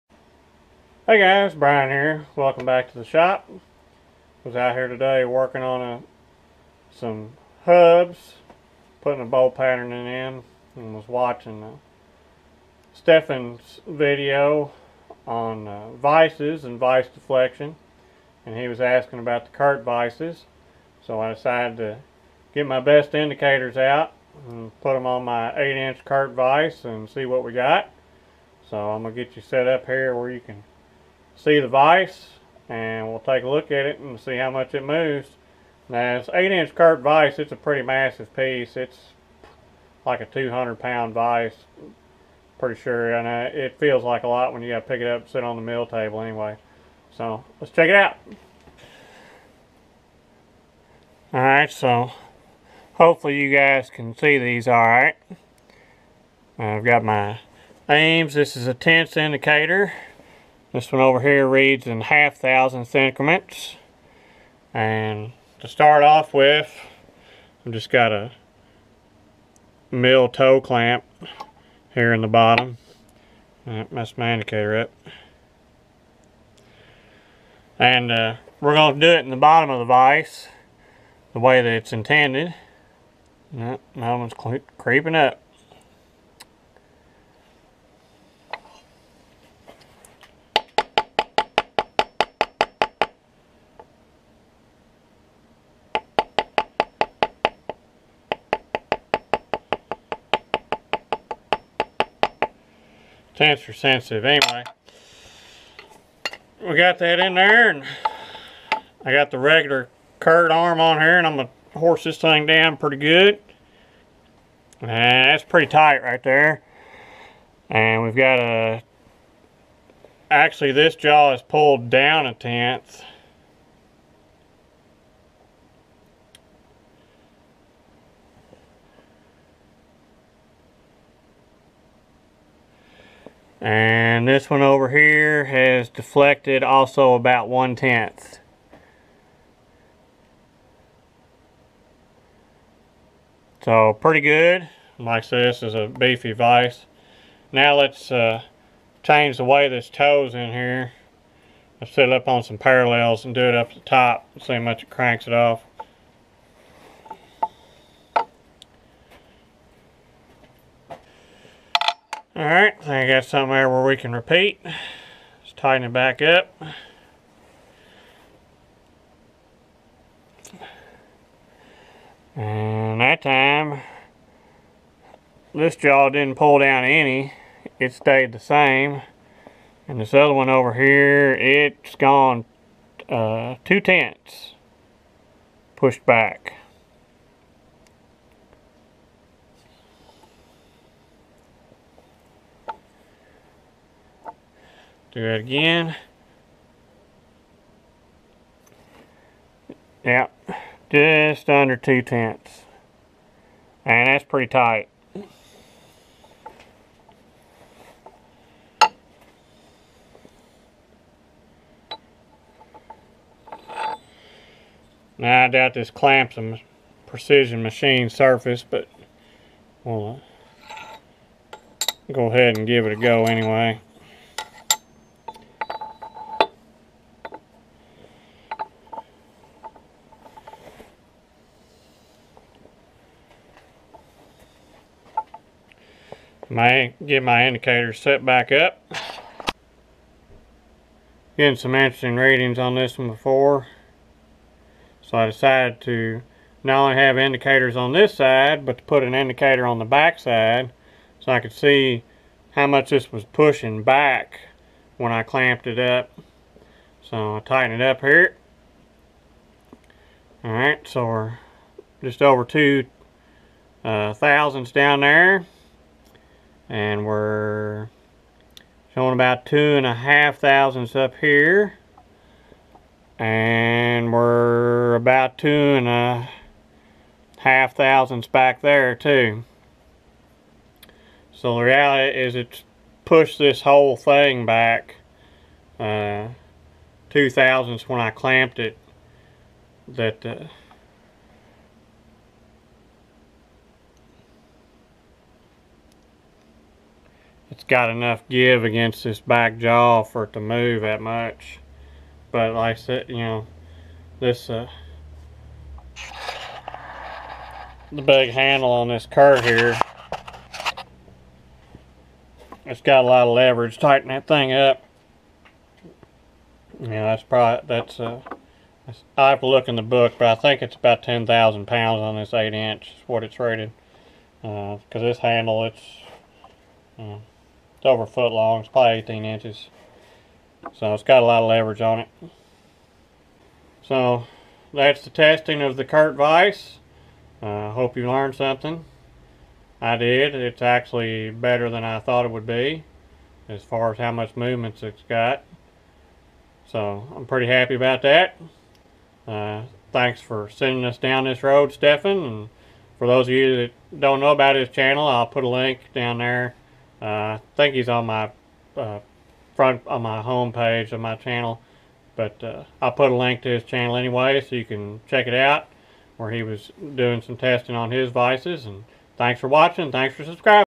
What is the deflection in my Kurt milling vise?
This is a video reply to Stefan Gotteswinter measuring the deflection in my Kurt vise which is an older D80 8" model.  I was very surprised that the big Kurt had similar movements to the pull down grinder vise especially considering the difference in clamping force.  I suspect the massiveness of the vise plays a large role in the minimal deflection.